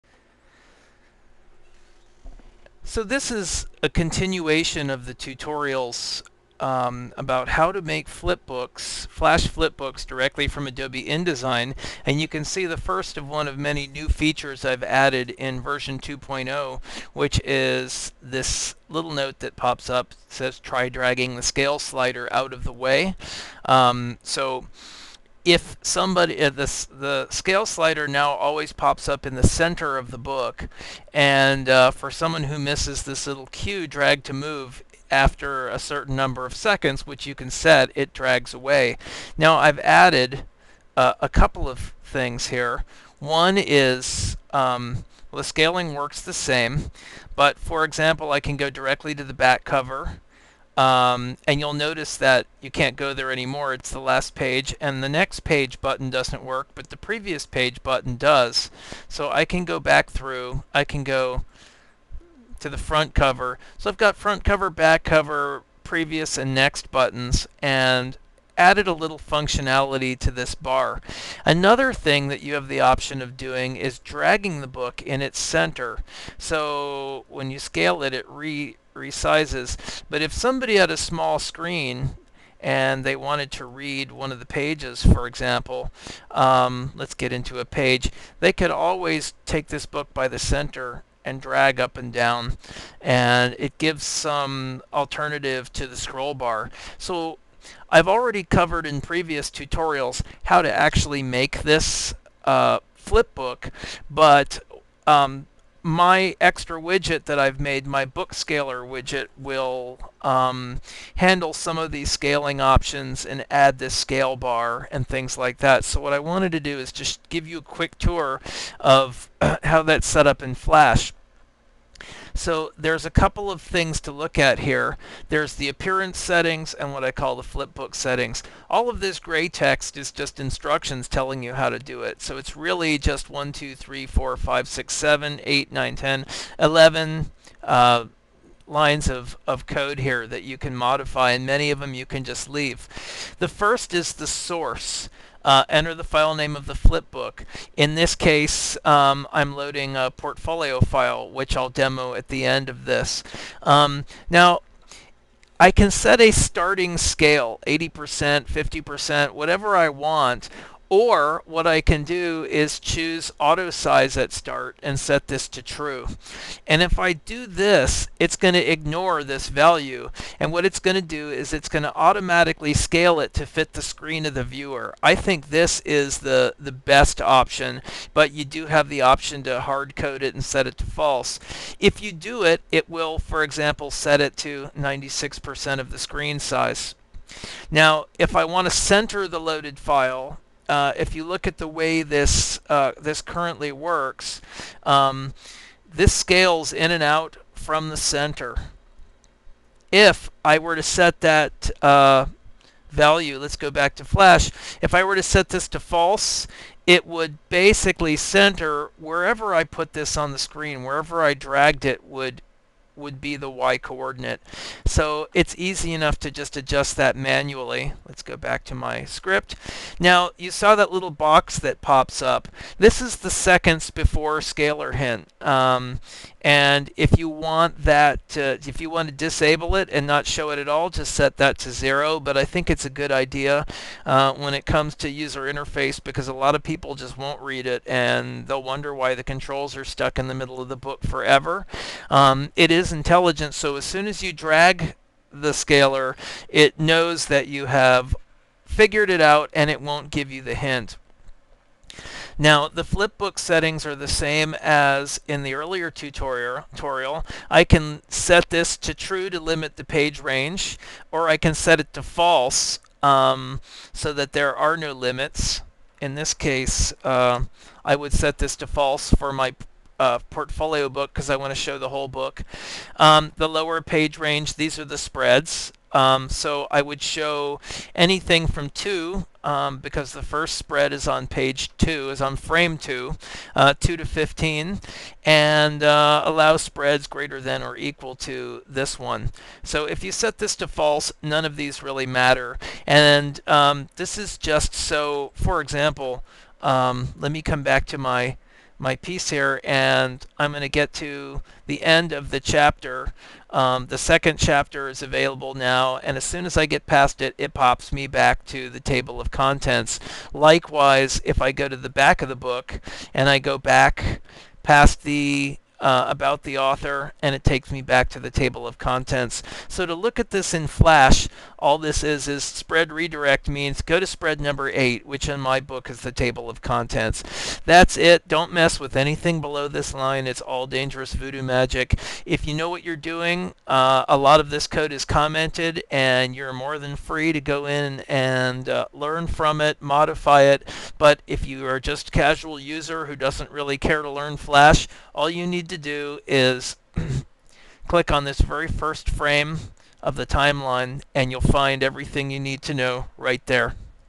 Exporting Flash Flipbooks from inDesign CS5 - Part 6 (1080p)
Learn how to export beautiful flash flipbooks directly from inDesign. My own book scaling feature is free to download and you you can easily modify it in flash. Part 6 shows you how to use and adjust the new features I've added to the bookscaler within the Flash editing environment. If you want to use the easy-XML version, skip to Part 8 of the tutorial. (Part 7 covers the interactive capabilities of Adobe InDesign).